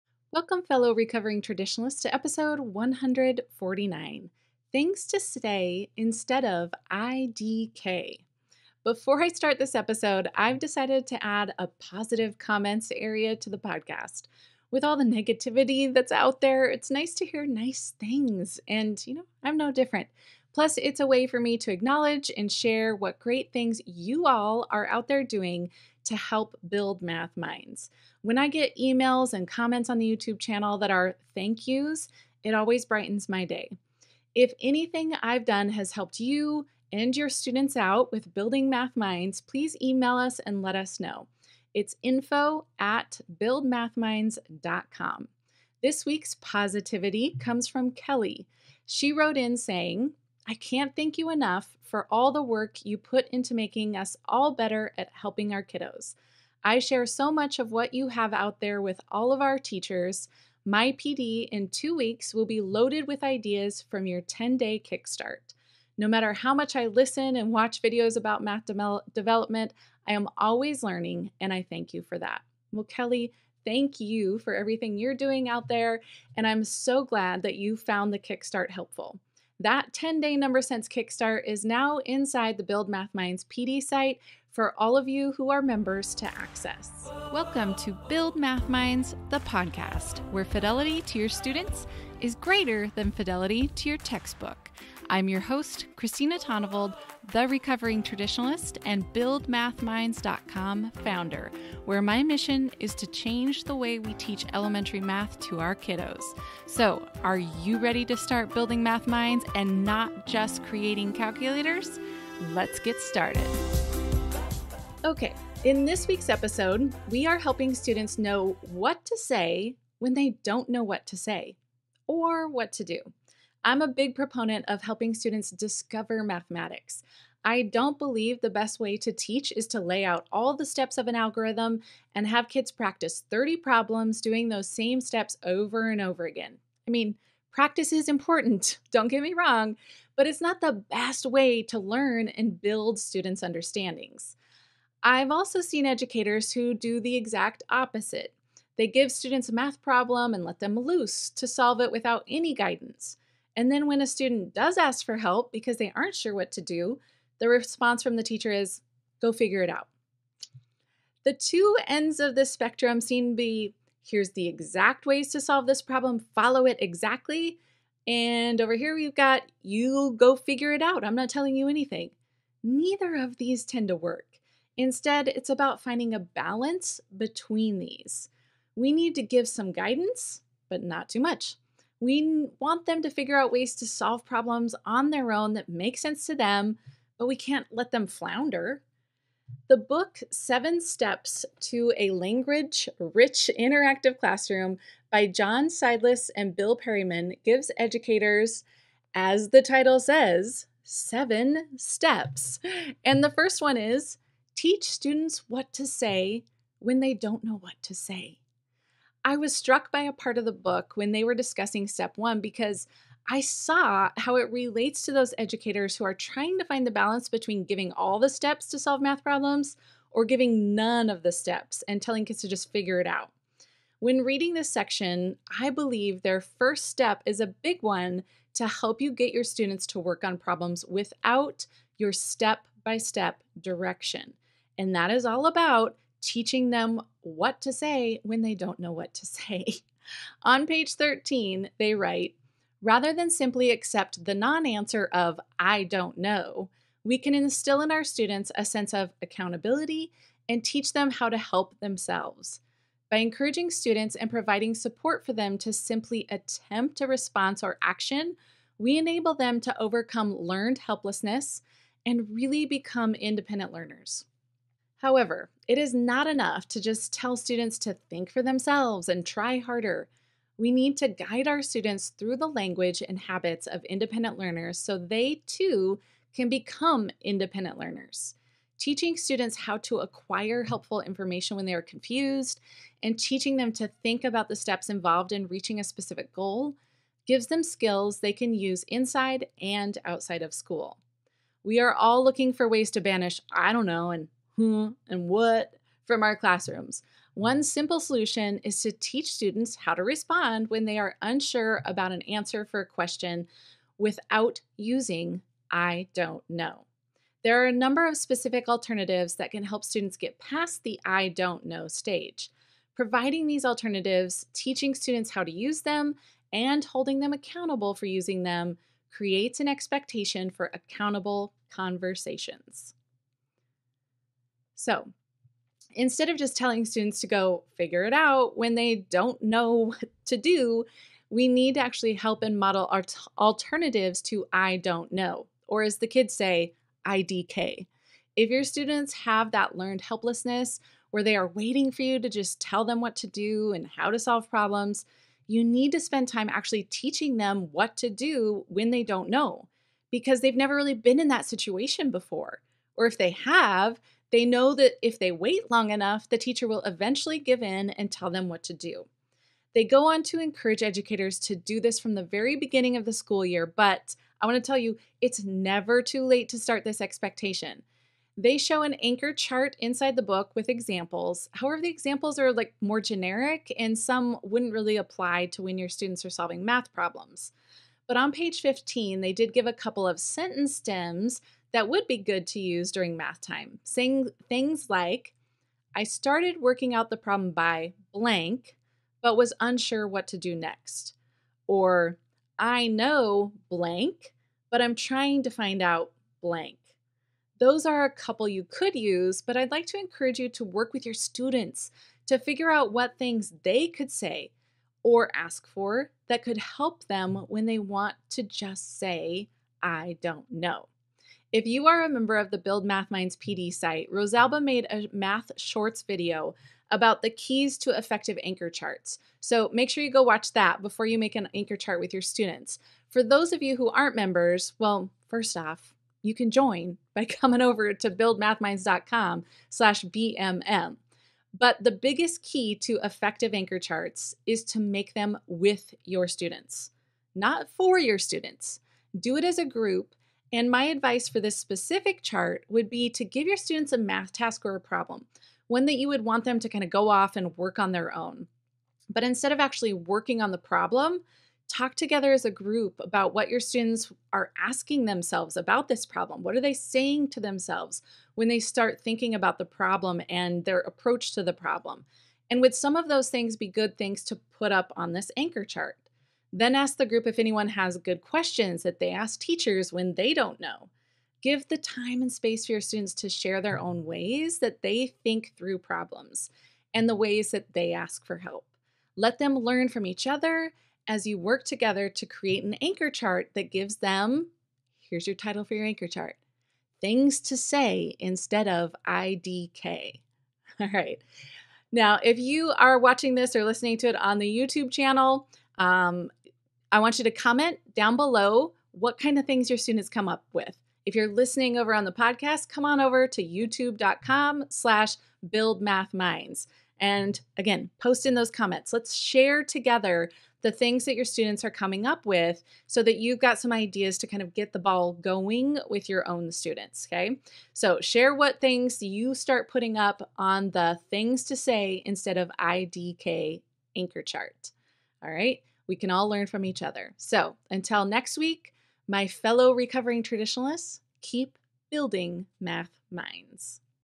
Build Math Minds Podcast #149 - Things to Say Instead of IDK
The two ends of the spectrum when teaching math seem to be “here are the exact ways to solve this problem, follow it exactly,” and “you figure it out, I’m not telling you anything.”  Neither of these tend to work.  Instead it’s about finding a balance between these. We need to give some guidance, but not too much.  We want them to figure out ways to solve problems on their own but we can’t let them flounder.

The book 7 Steps to a Language-Rich, Interactive Classroom by John Seidlitz & Bill Perryman gives educators, as the title says, 7 steps and the first one is: teach students what to say when they don’t know what to say. This also applies when a student doesn’t know where to start with a math problem.  So this week’s episode is helping students have Things to Say Instead of IDK.

The book mentioned in this video: 7 Steps to a Language-Rich, Interactive Classroom by John Seidlitz & Bill Perryman